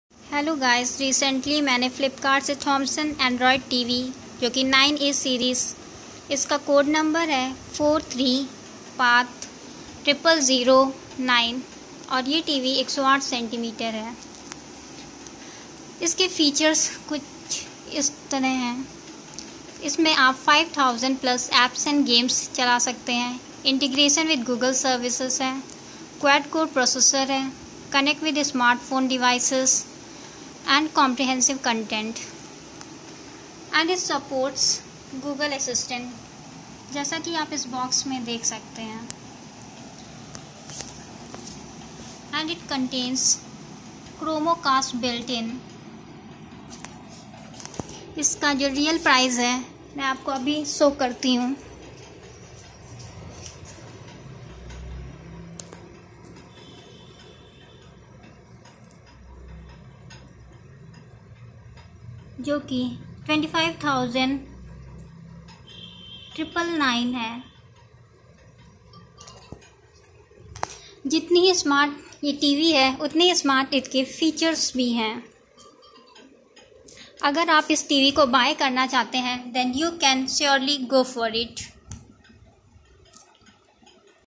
Thomson TV
Review 43inch(108cm) Android LED TV with HD quality